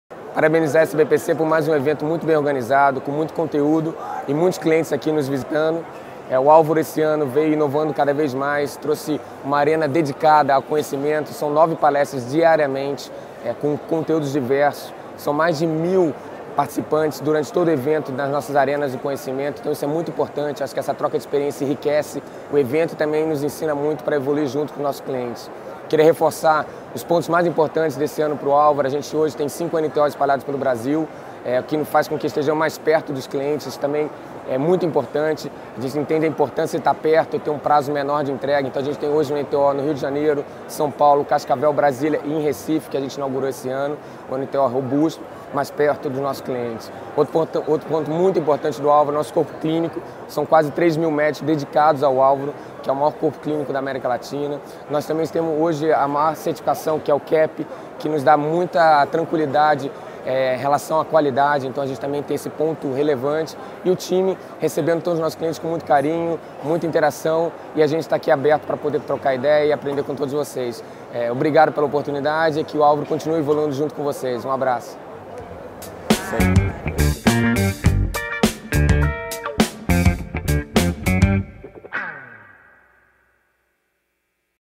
Alvaro Apoio recebe clientes em seu estande no 53º Congresso Brasileiro de Patologia Clínica
O 53º Congresso Brasileiro de Patologia Clínica/Medicina Laboratorial aconteceu no Rio de Janeiro, de 24 a 26 de setembro de 2019